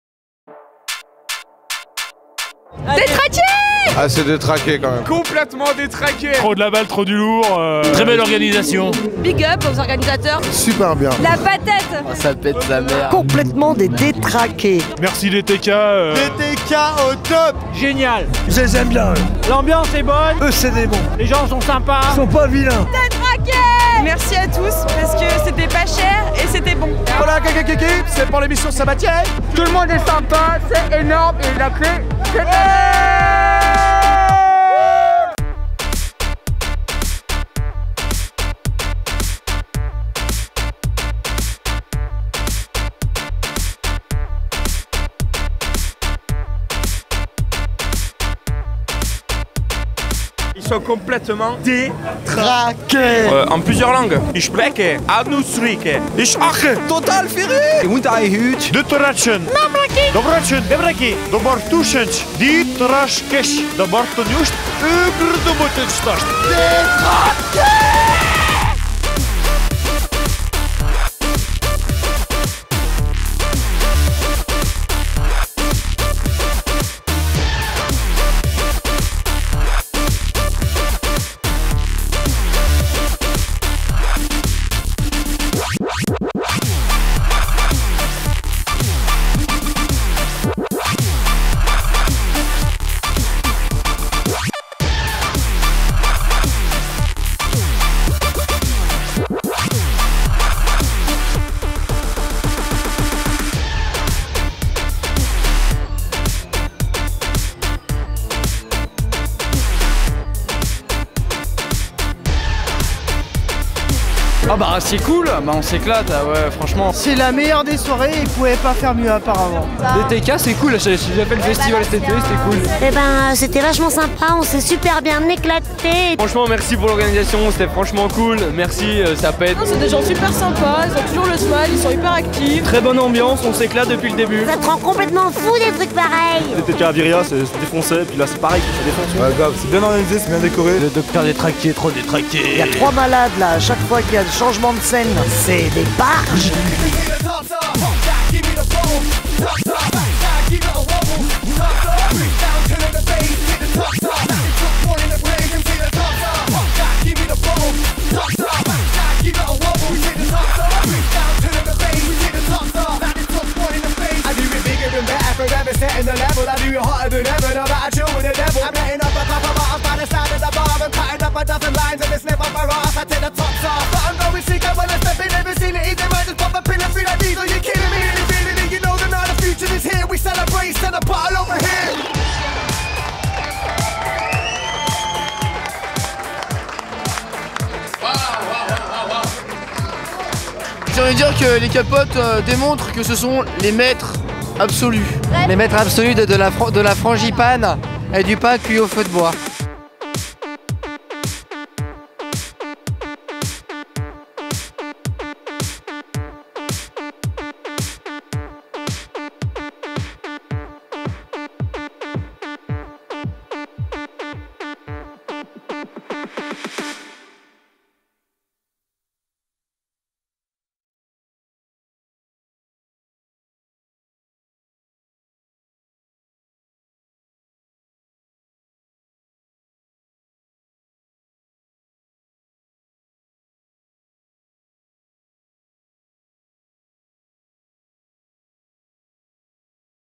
Les Folles Expériences du Dr. DéTraKé #1 - Aftermovie
"Les Folles Expériences du Dr. DéTraKé"

AVEC : Ced Soul Session (Lyon) / Azad Lab (Toulouse) / L'animalerie (Lyon) / D-Turn (Genève) / Darkham (Bourg-en-Bresse) / Lowkey & Kardinal (Bourgogne) / L-xir (Pontarlier) / Zef (Jura) / Les Crock-Mort du Rock (Bourg-en-Bresse)

SPECIAL GUEST : Dr. DéTraKé !!!

Installation "MagiK 'Zombies' Forest" - Collectif OPLA (Villeurbanne)

La Ferme à Jazz (01) - 05/01/2013

--- Association K-potes ---
--- K-potes ProduKtions ---